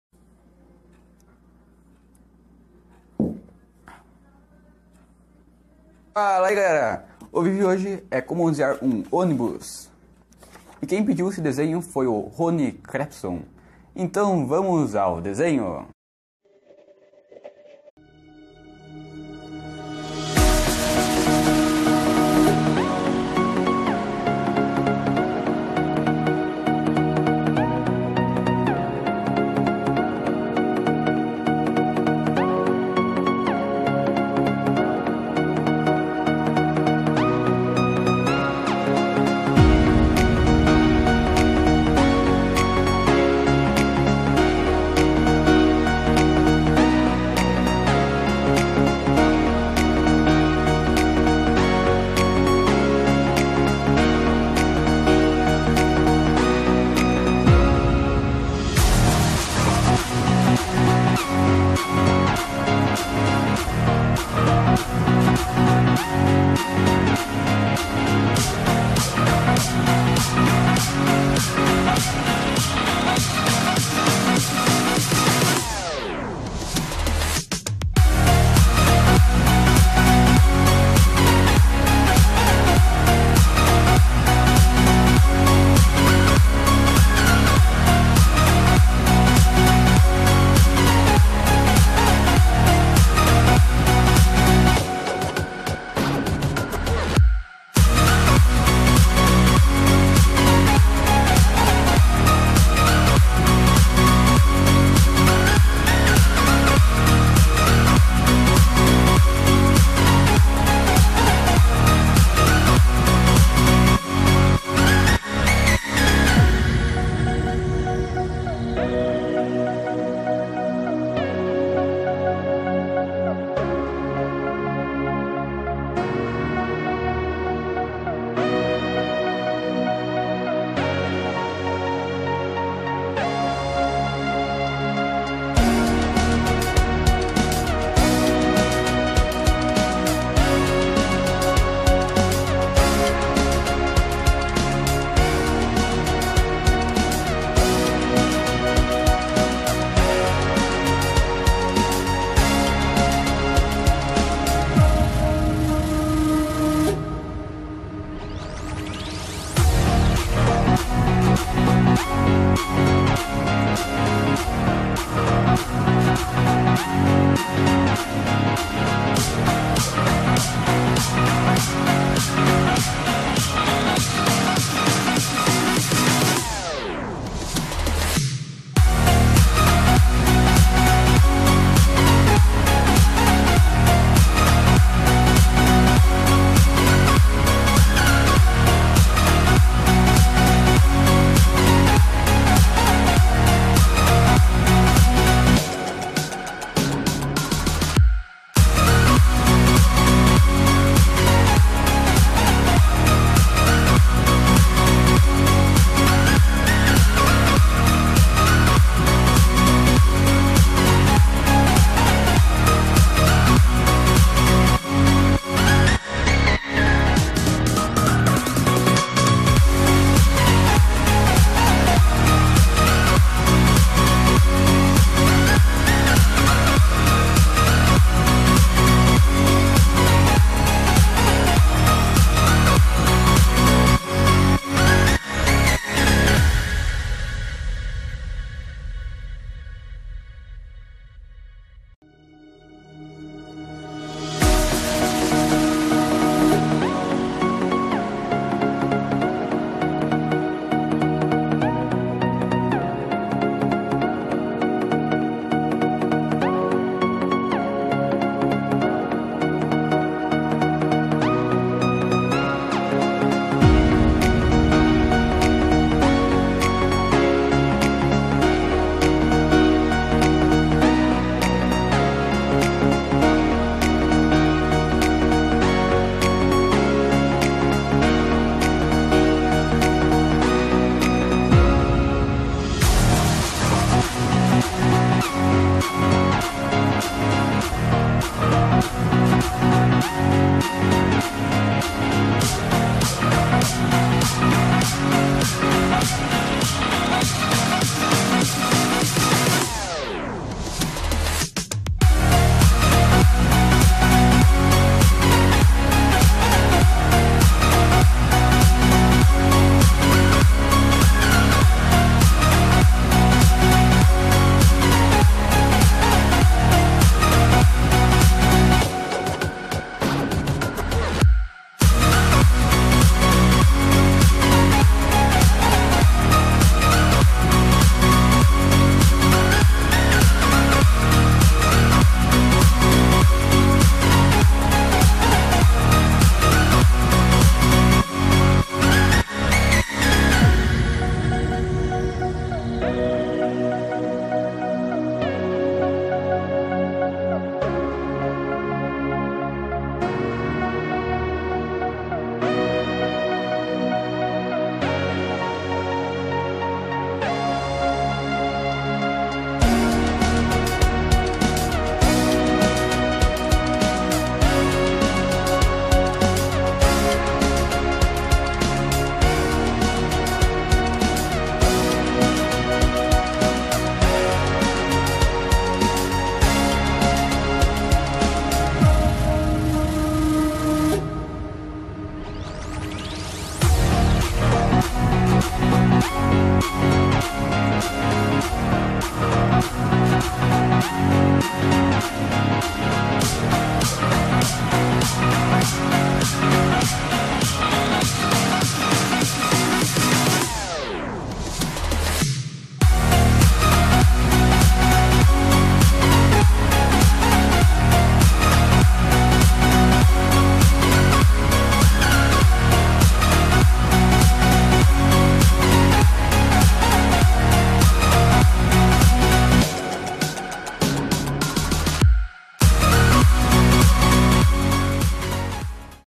Como desenhar um ônibus!!!
Falai galera! Este video , foi Rene Klepson que pediu,se gostar curta, não se esqueça do seu like, desenhos que vocês querem deixem nos comentários!  E desenhos como esse você encontra aqui no Desenha Tudo !!!